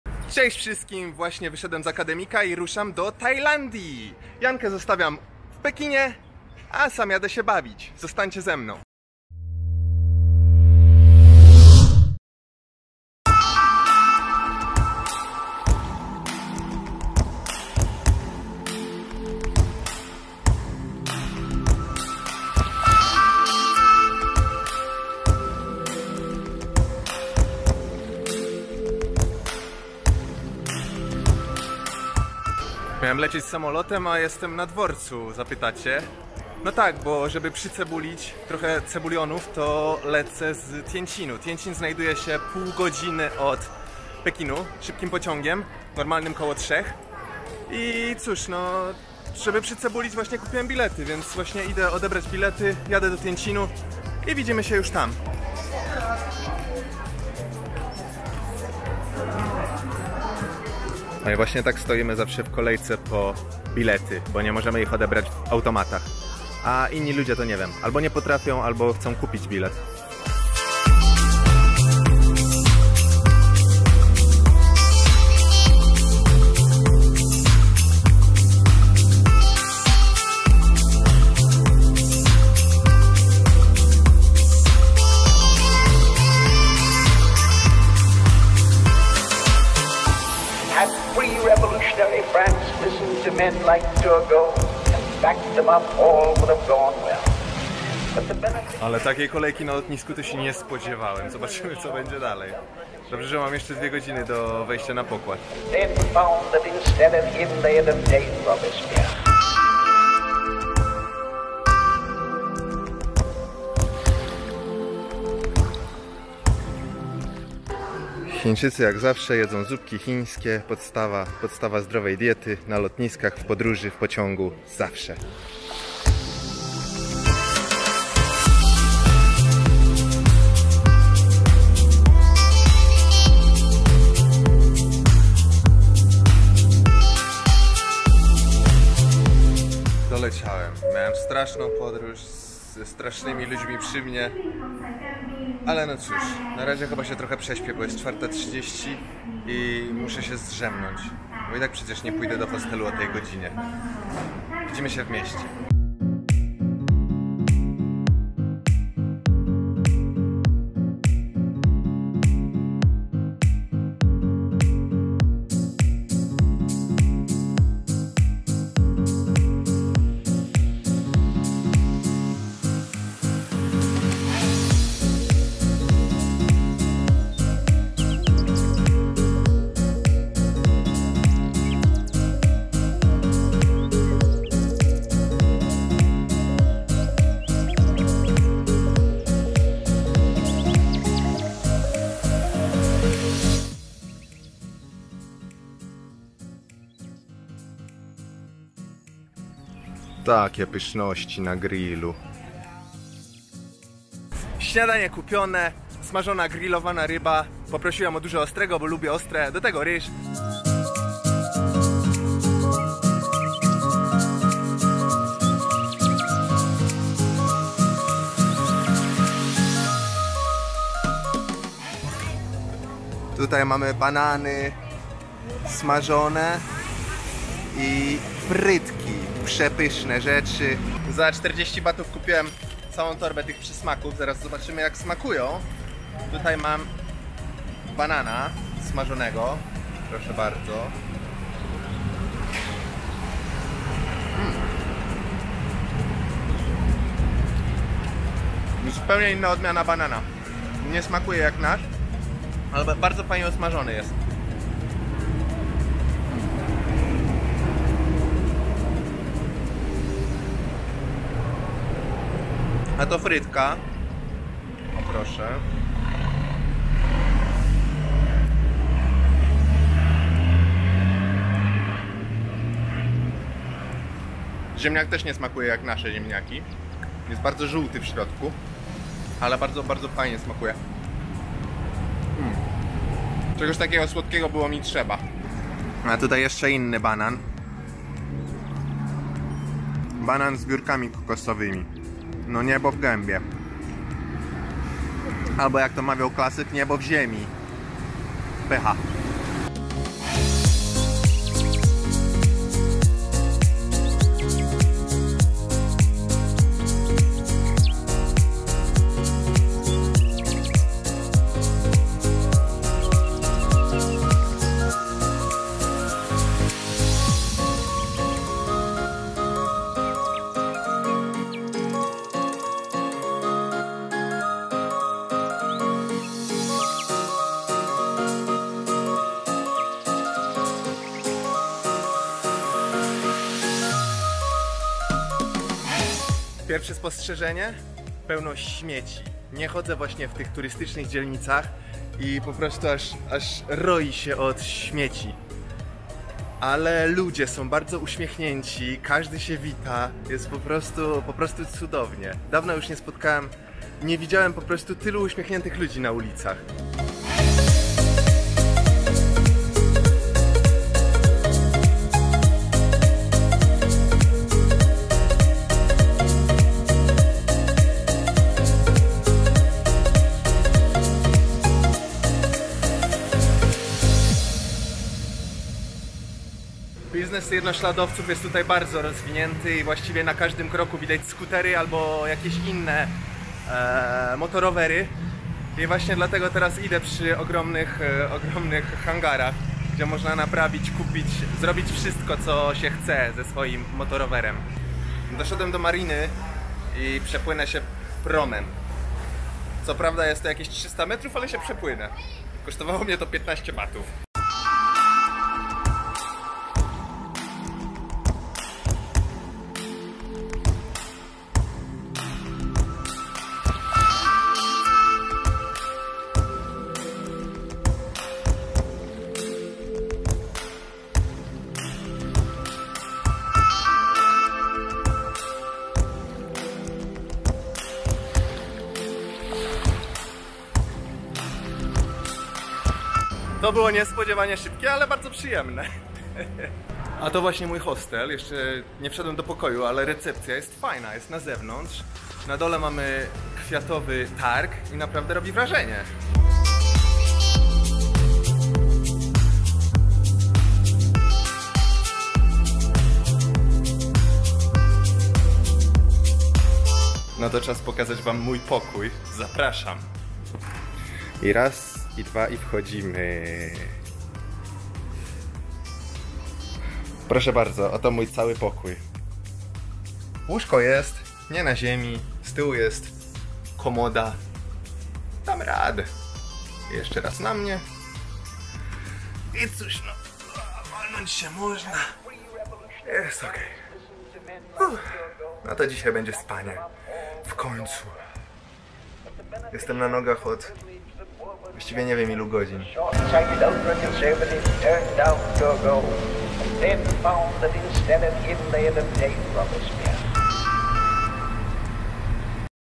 Droga do Bangkoku była długa i wyboista!
Nowa seria na kanale, tym razem... BANGKOK! Cały film to taki swoisty wstęp, w którym pokażę Wam, jaką trasę pokonałem, żeby oszczędzić trochę cebulionów i nie lecieć najdroższym samolotem z Pekinu. Wspólnie rzućmy okiem na pociąg podmiejski jadący z lotniska do Bangkoku, a także na pierwsze streetfoody!